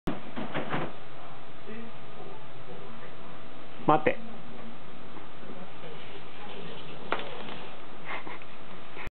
アメリカンコッカー 蘭々ちゃん♪ NG編 待ってますがねパート１.MOV
蘭々だよ♪「待て！」ってパパが言うから待ってるんだけど・・・